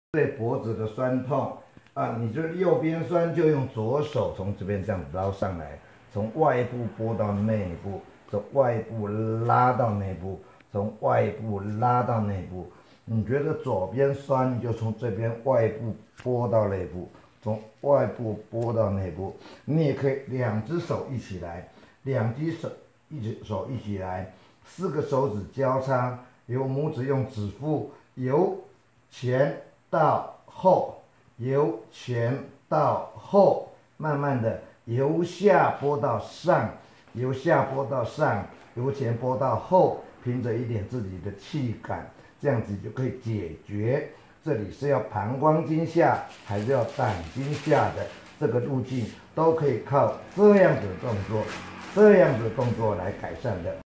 改善脖子的痠痛
同時剝到膽經的"風池"與膀胱經的"天柱"
參考